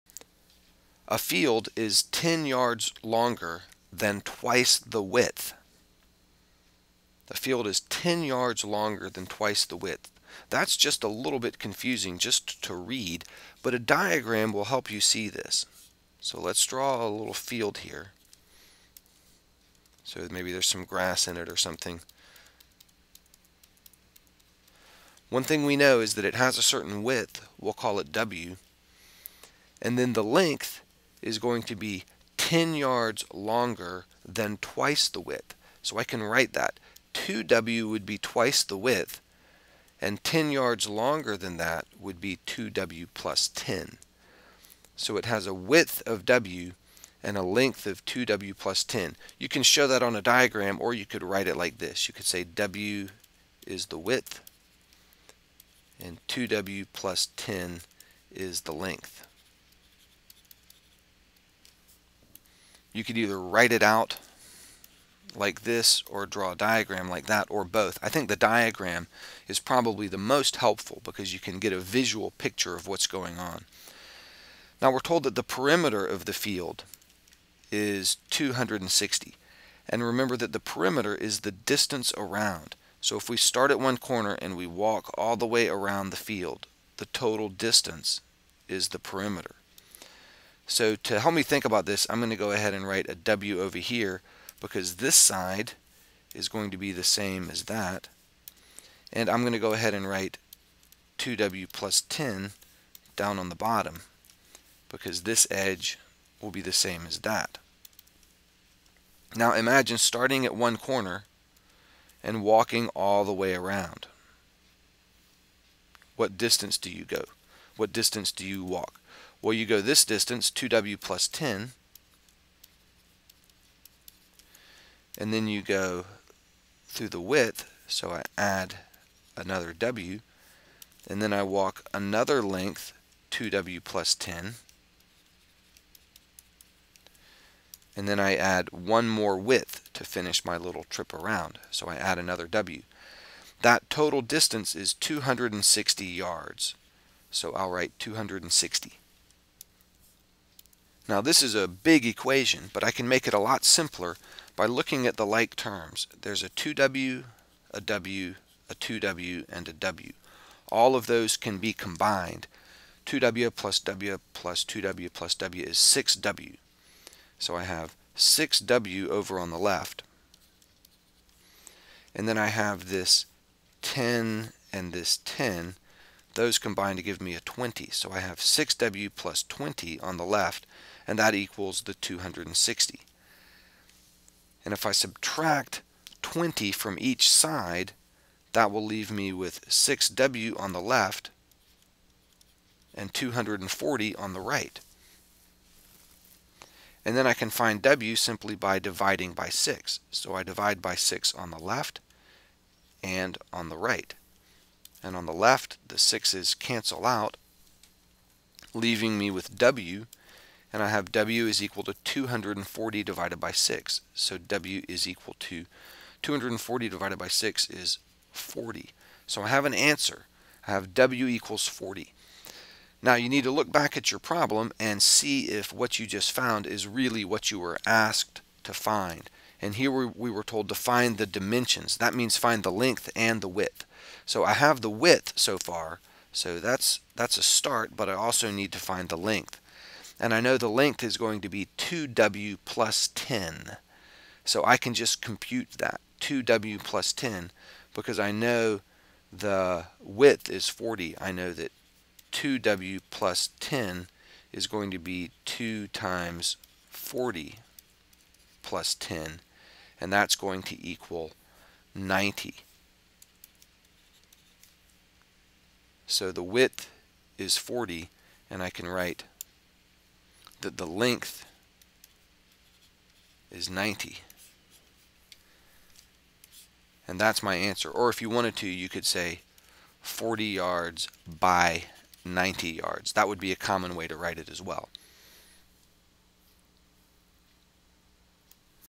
Prealgebra 4.6f - Example
The problem:  A field is 10 yards longer than twice the width.  The perimeter of the field is 260 yards.  Find the dimensions of the field.  The problem is set up, explained, and solved.